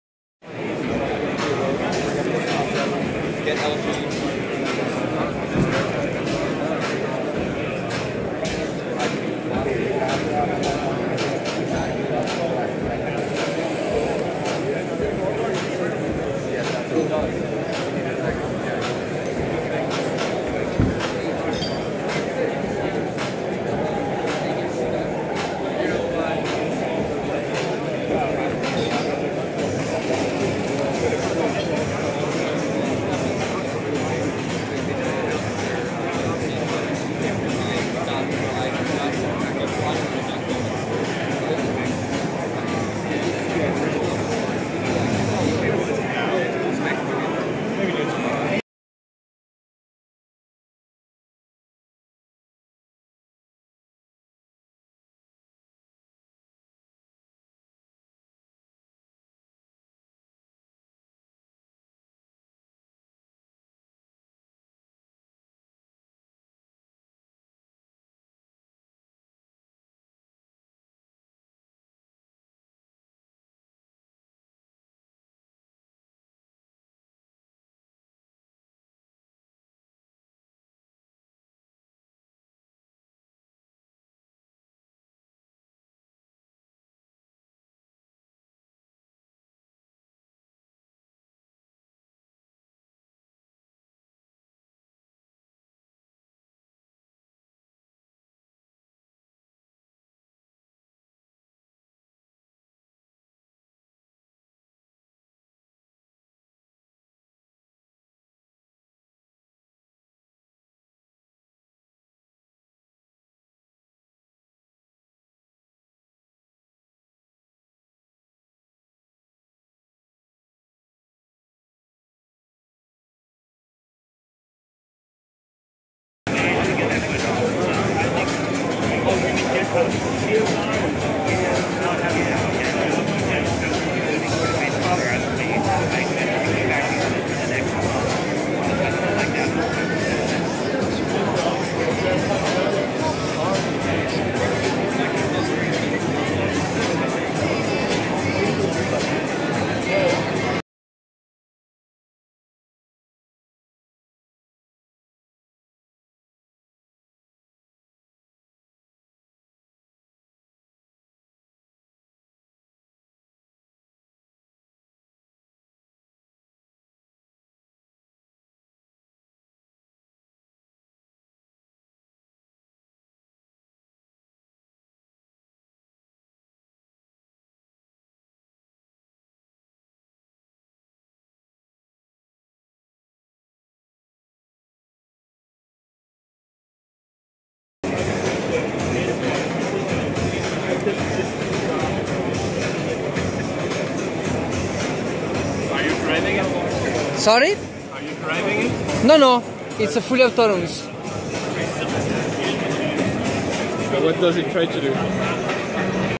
MyzharBot - Autonomous Wandering @GTC2015 #5
In this video MyzharBot is wandering around the people attending GTC2015 at the Nvidia booth in San Jose, California.